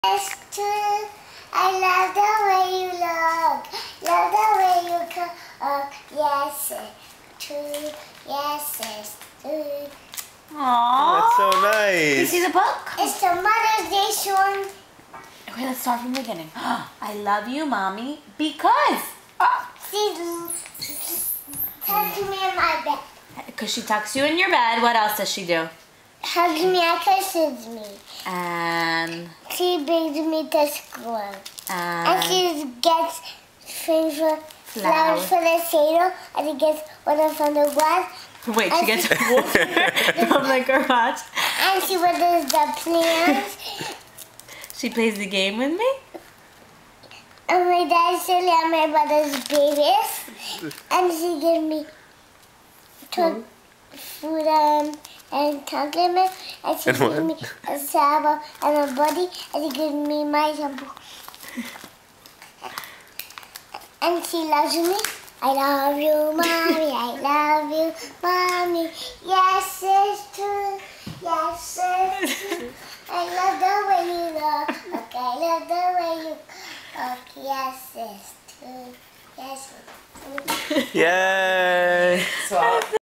Mimi's Mothers Day Song
Mimi Feinberg sings a mothers day song